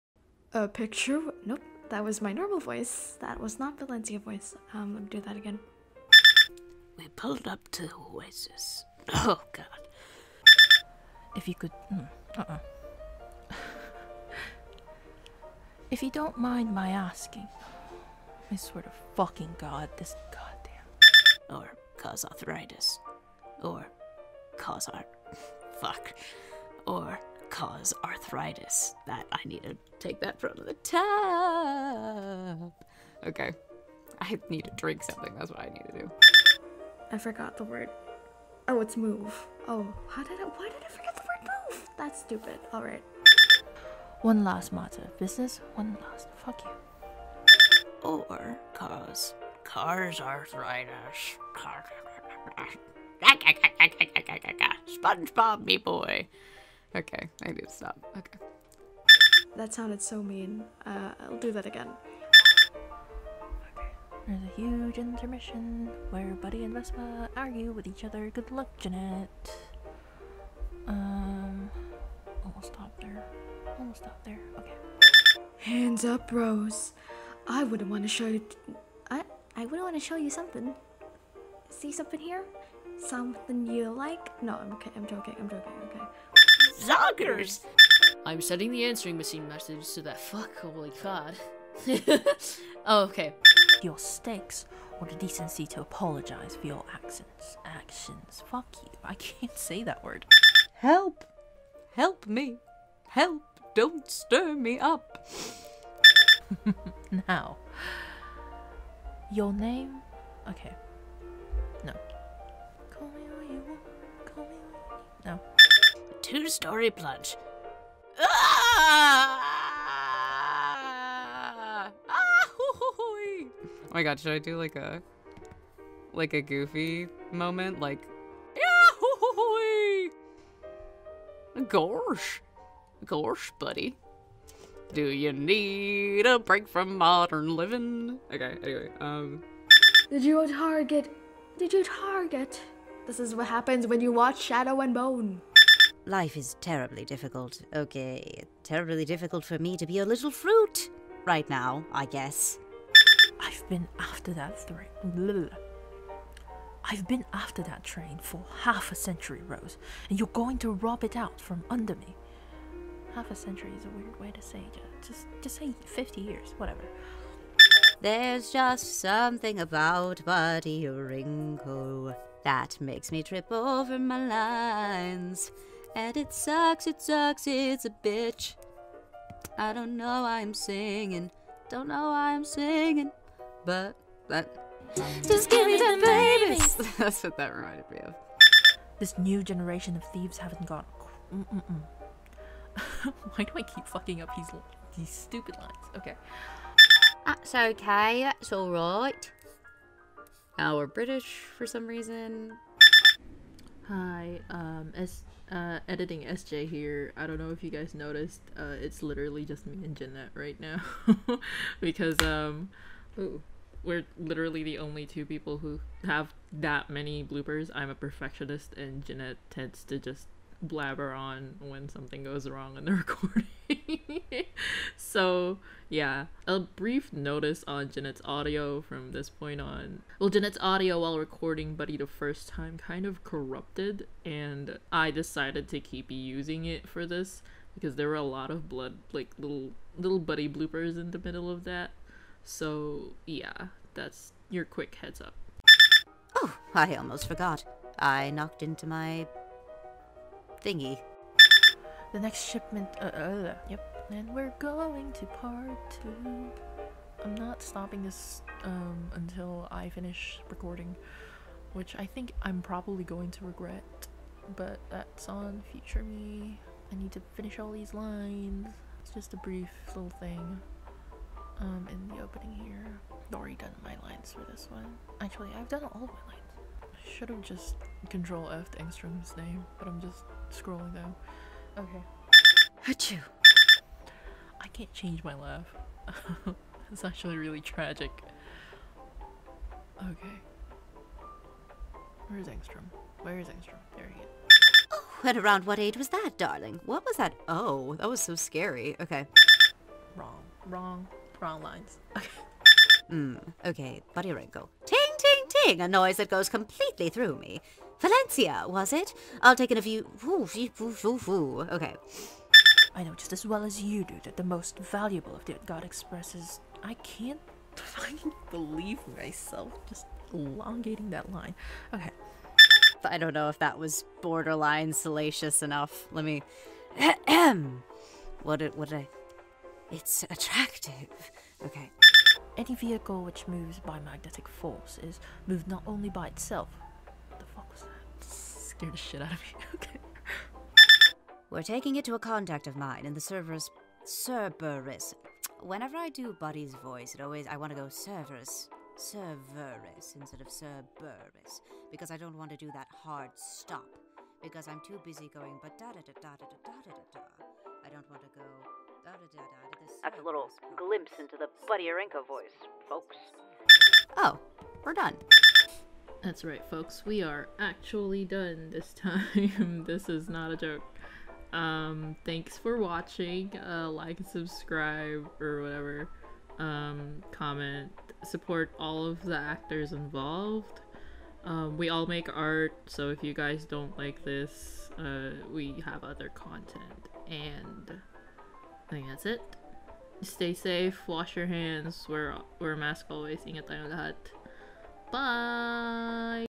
RBG BLOOPERS?!
Yep, here we go with another round. With references, weird accents, actors getting jumpscared by errant sounds of airconditioning, and lessons on Buddy Aurinko.

All actor art used in this video apart from the outro one was made by Jeannette!

You can support the cast and crew by following them on social media and/or donating...

For announcements and bonus content (read: memes), follow the AU on the official Twitter account